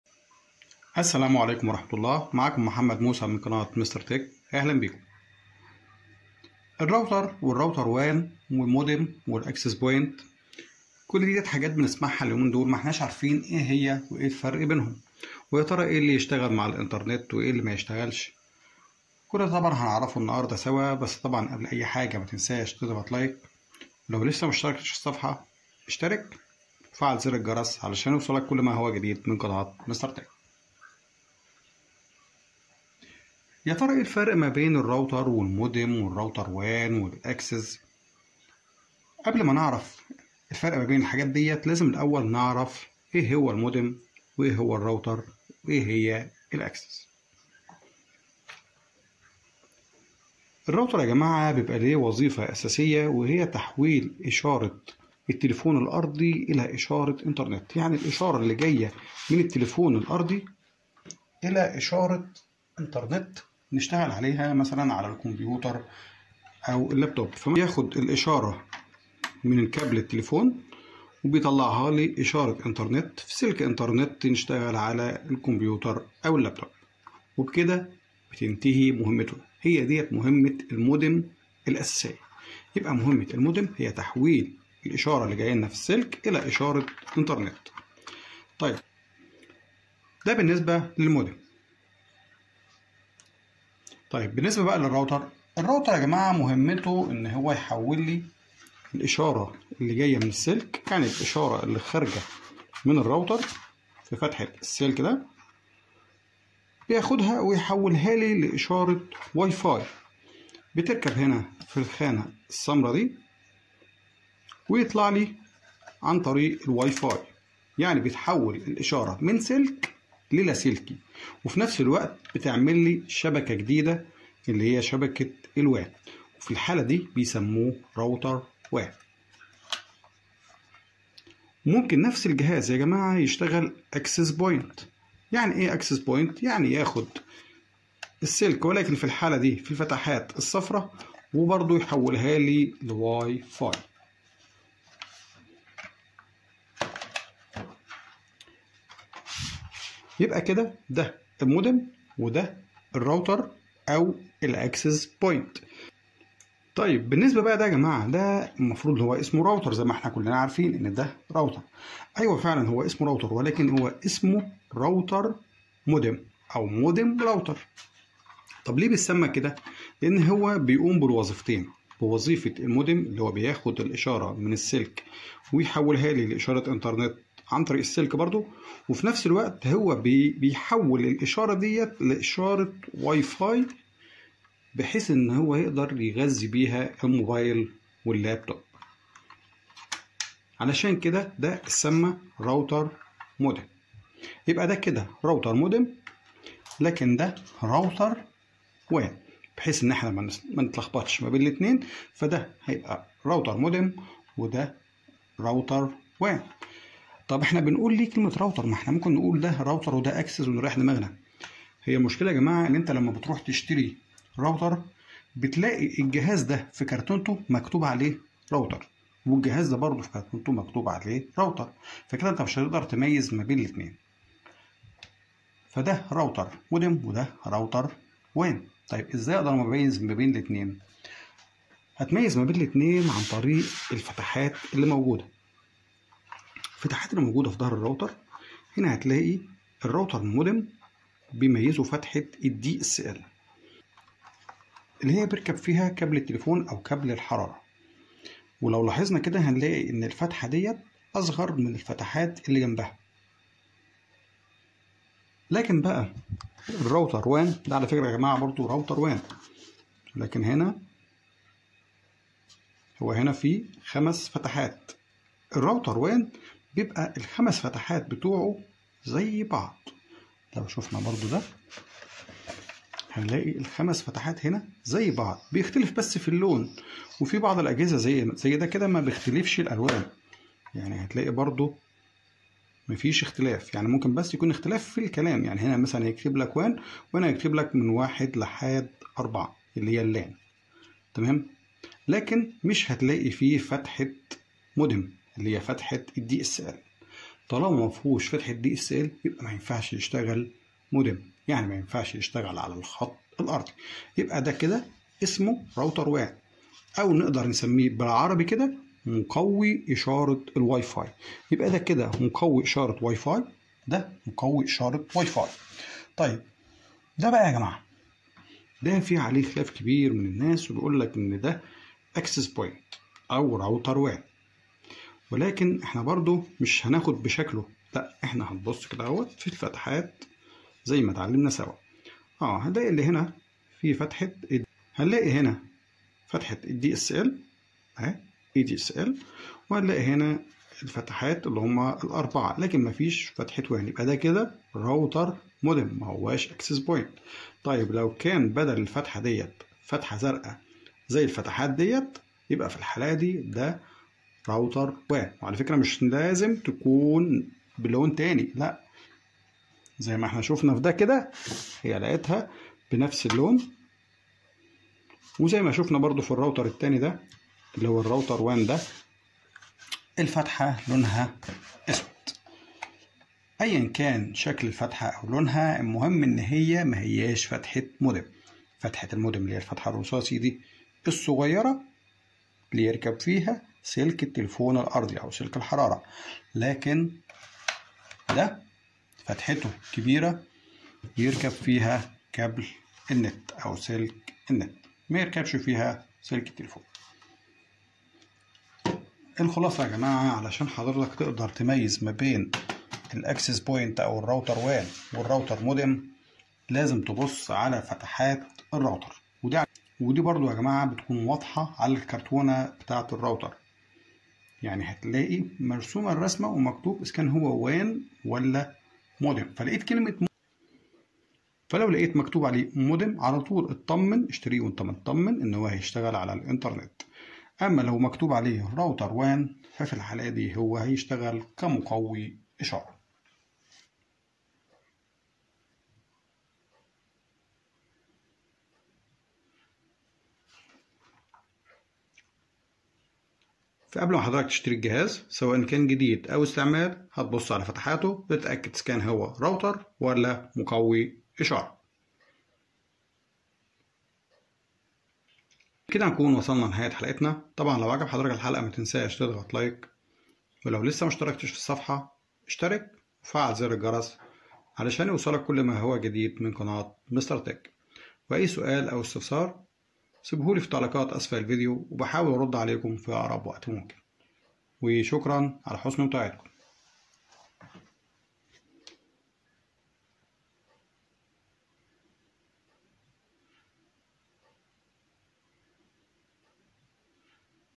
الروتر والروتر وان - والفرق بين الروتر والاكسيز والموديم Router Wan - Router modem - Access
الروتر والاكسيز والموديم والوان وايه الفرق بينهم
وايه إلى يشتغل على النت الارضى